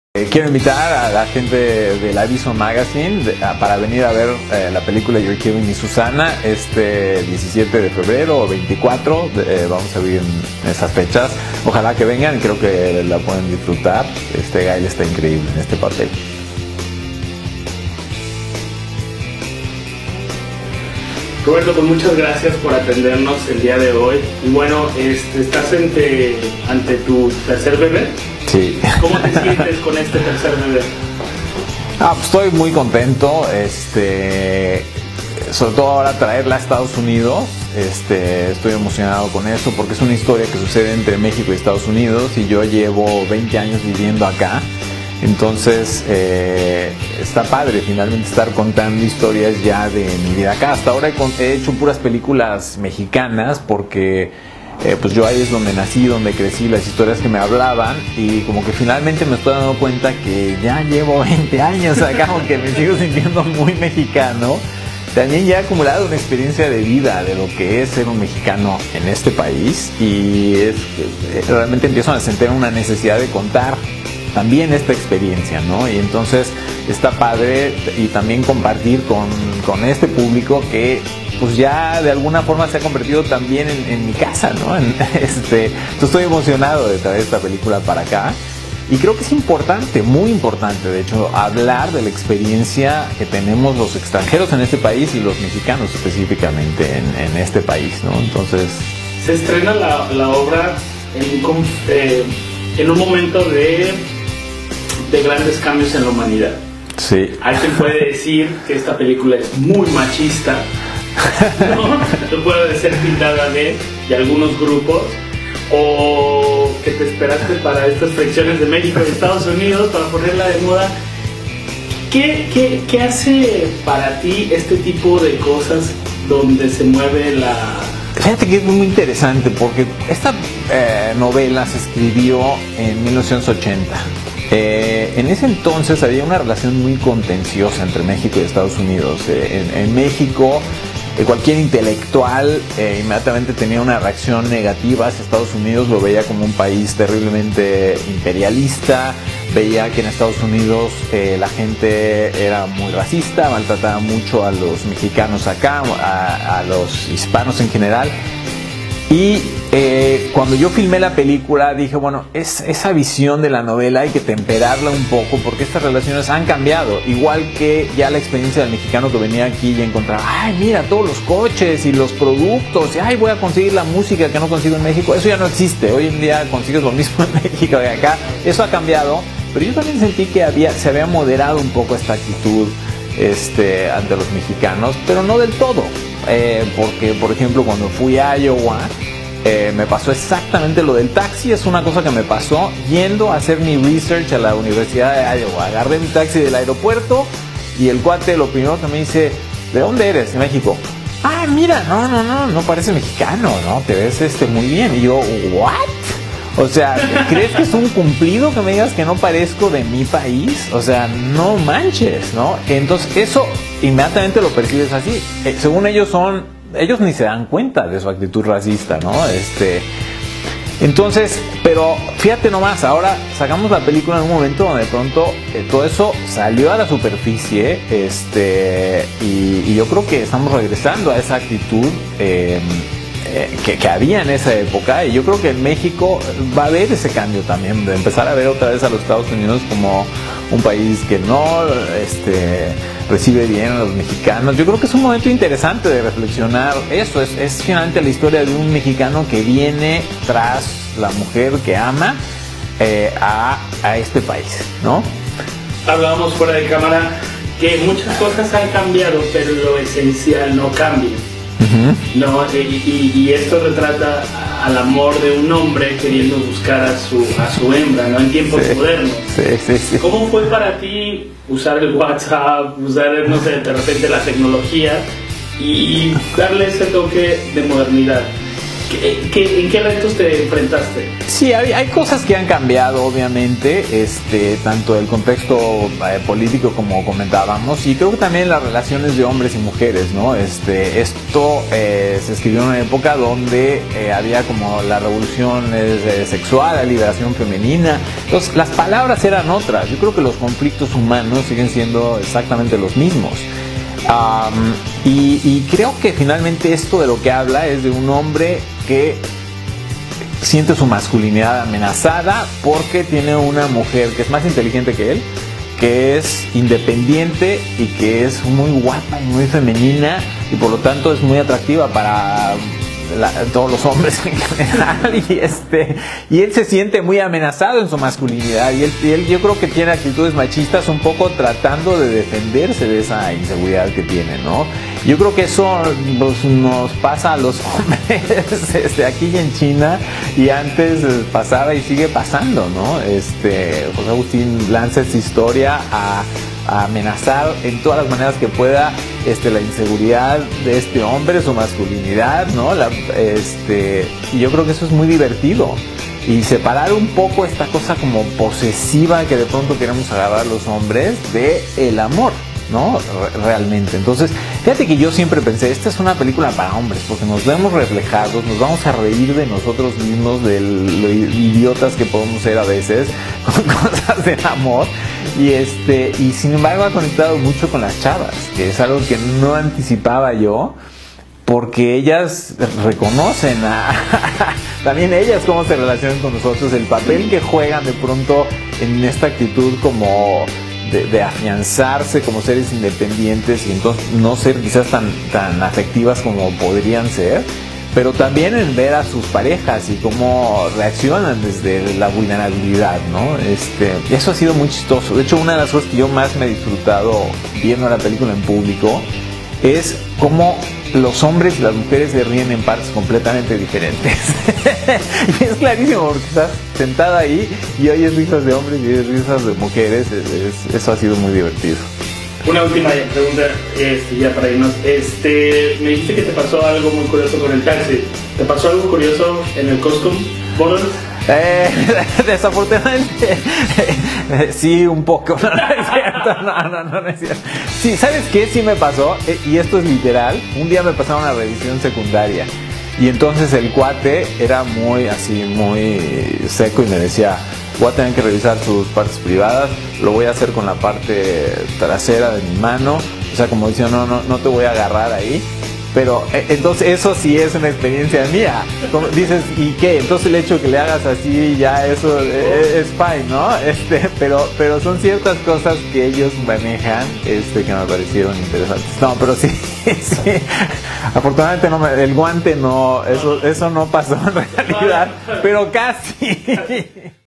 El director de You're Killing me Susana en entrevista
Roberto Sneider nos cuenta todo sobre su nuevo filme